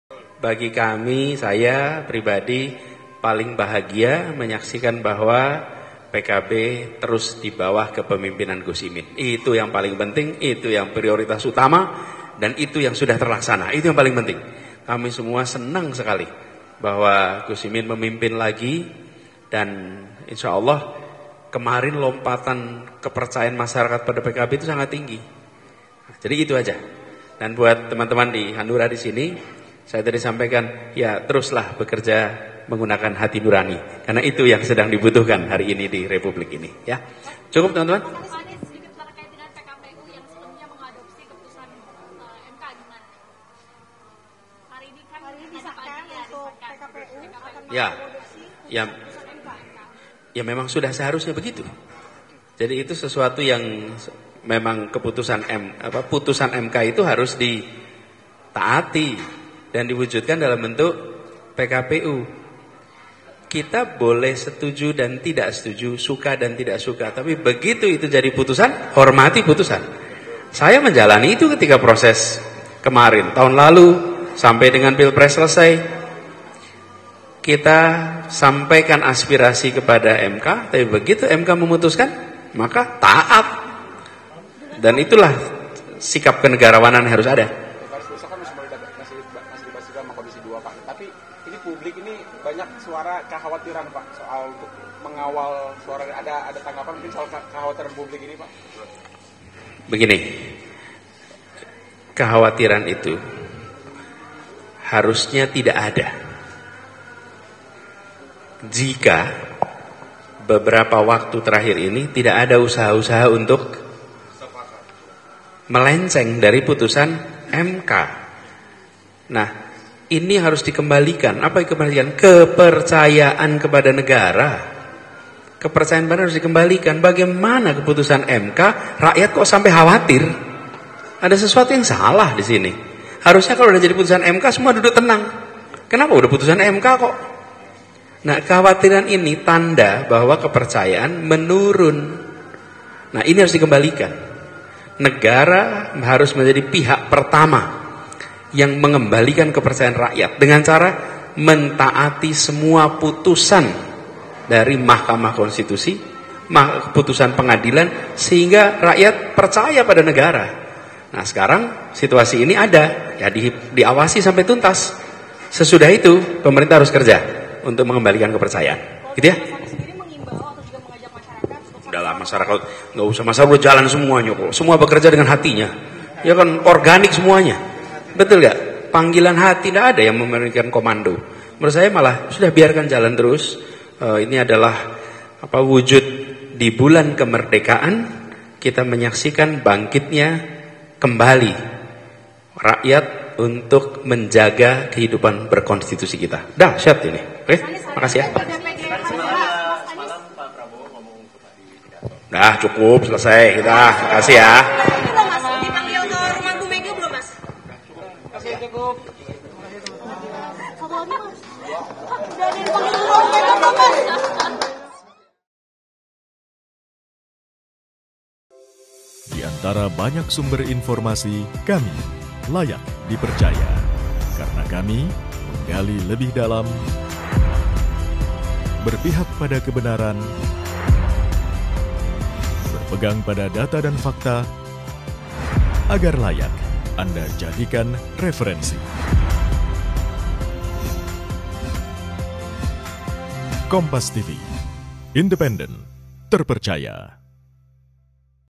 Anies Respons Cak Imin jadi Ketum Lagi hingga Singgung Penolakan Revisi UU Pilkada usai Putusan MK
KOMPAS. TV - Eks Gubernur DKI Jakarta, Anies Baswedan, menyatakan kegembiraannya atas terpilihnya Muhaimin Iskandar sebagai Ketua Umum Partai Kebangkitan Bangsa (PKB). "Saya paling bahagia melihat PKB tetap di bawah kepemimpinan Gus Imin, itu yang paling penting," ujar Anies di Menteng, Minggu (25/8/2024).

Anies juga menanggapi suara publik yang menolak revisi UU Pilkada dan mengungkapkan kekhawatirannya terkait Putusan Mahkamah Konstitusi (MK). Menurutnya, kekhawatiran ini seharusnya tidak ada jika tidak ada upaya yang melenceng dari putusan MK. Anies menekankan pentingnya mengembalikan kepercayaan publik dan memastikan pengawasan hingga tuntas.